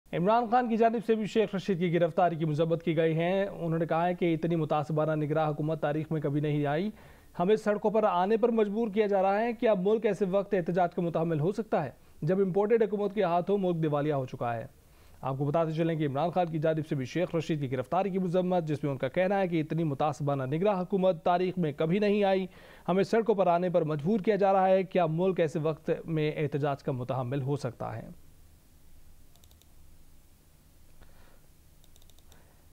Breaking News - Imran Khan's reaction to the arrest of Sheikh Rasheed - PTI - Awami Muslim League
In a major development, Awami Muslim League (AML) chief Sheikh Rashid Ahmed — a close aide of Pakistan Tehreek-e-Insaf (PTI) Chairman Imran Khan — was arrested from Murree Motorway in the early hours of Thursday, police confirmed.

The former interior minister and his nephew, Sheikh Rashid Shafiq, however, contradicted the police's version and said that took him into custody from his house and not the motorway.

According to the police, a bottle of liquor and a weapon has been recovered from Sheikh Rashid's possession. Police also said that the former railway minister was intoxicated.

PPP's Rawalpindi Division President Raja Inayat-ur-Rehman had filed a case against the former federal minister in Islamabad's Aabpara police station for hurling allegations at former president Asif Ali Zardari for hatching a plot to assassinate Pakistan Tehreek-e-Insaf (PTI) chief Imran Khan.

Sources told Geo News that the AML chief would be handed over to the Islamabad police.

Rashid's Lal Haveli has also been in the limelight after the Evacuee Trust Property Board (ETPB) sealed seven units including two units of Lal Haveli and five adjoining units on January 30.
Sheikh Rashid talks to media after arrest

Talking to the media after his arrest, Sheikh Rashid said, "Police thrashed up his children. I was wronged."

He said that at least 100 to 200 armed people barged into his house with the help of ladders, broke the doors and windows of the house, beat his servants and searched his residence.

The former interior minister said police forcibly hustled him into their vehicle. He said the police arrested him despite the fact that Mian Tahir 's court granted him bail and ordered the IG to appear before the court on February 6.

He said that the interior minister Rana Sanaullah is behind all what is happening. He, however, said that truth will prevail at the end of the day and he stands by Imran Khan.
Imran condemns arrest

PTI Chief Imran Khan took to Twitter to condemn the arrest of his close aide and AML chief and saying he "strongly condemns" his arrest.

The former premier added that "never in our history have we had such a biased, vindictive Caretaker govt appt by totally discredited ECP. "Question is can Pak afford a street movement which we are being pushed towards at a time when Imported Govt has bankrupted us?" he questioned.
The FIR

In the FIR, the PPP divisional president stated that the AML chief tried to badmouth a former president and cause a "permanent danger" to the PPP co-chairman and his family. The FIR added that Rashid, with his provocatory allegations, wants to cause a fight between the PTI and the PPP and disrupt the country's peace.

The PPP leader added that the AML chief claimed to have vital information regarding a 'conspiracy' being hatched to assassinate the PTI chief. "If that's the case, then the police should take both the leaders into custody under Section 150 and 151 and fail the conspiracy" to stop the spread of unrest in the country, he added.